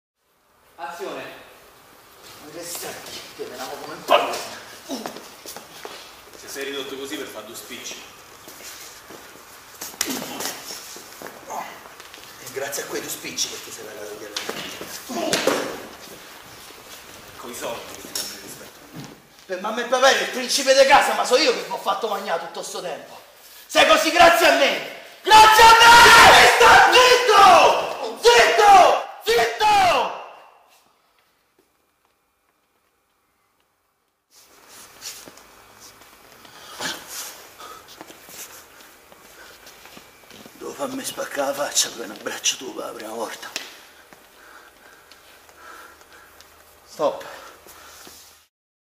Live Action Combat 2018
E' il momento del Fight Club in RFA! Esercitazione preparatoria ai set esterni per i ragazzi del secondo anno, con la supervisione dei docenti Alfredo Betrò, Kristoph Tassin e il mastro d'armi Francesco Lodà. Fight!